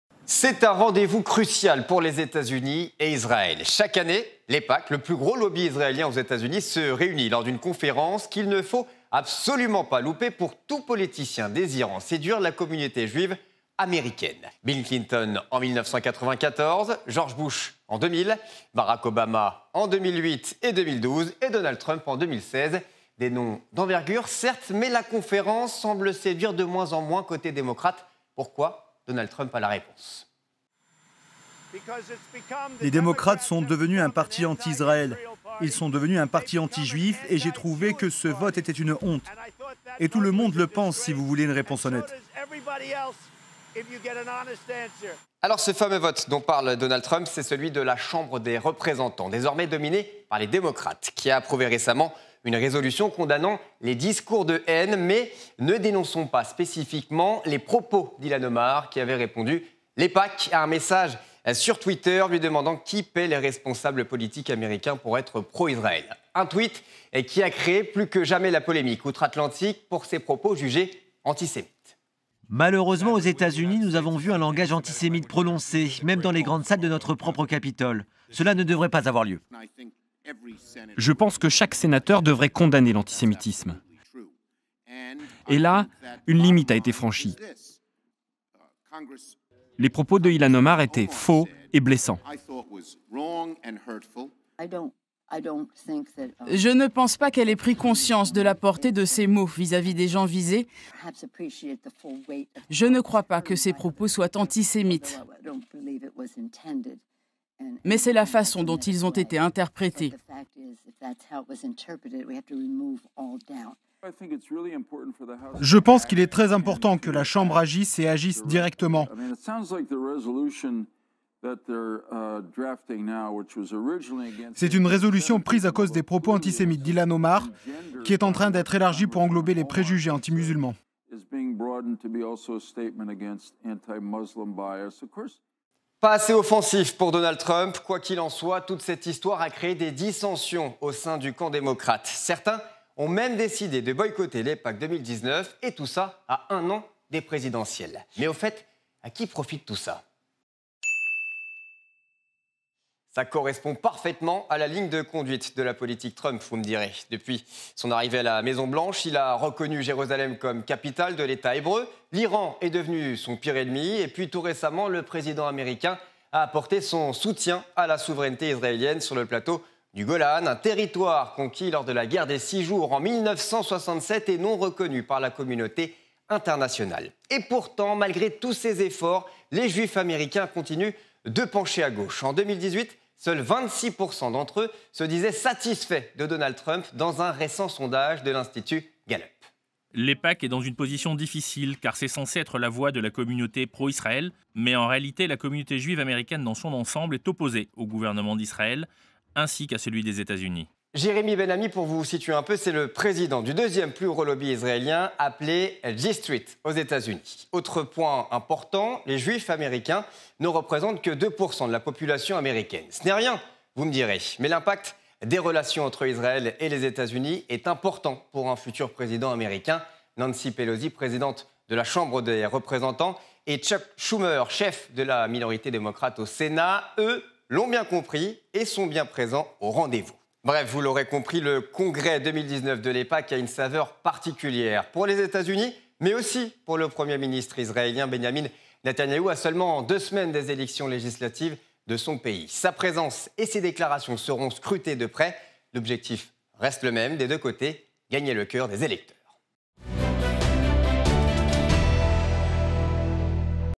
RT / L'AIPAC divise la classe politique américaine (26.03.2019)
Le congrès de l'AIPAC, lobby américain pro-Israël, vient de s'achever, alors même qu'est signée la souveraineté de l'Etat hébreu sur le plateau du Golan par Donald Trump. L'AIPAC a contribué pendant des décennies à assurer à l'Etat hébreu un soutien indéfectible des États-Unis. Mais depuis peu, il créé des divisions au sein du parti démocrate.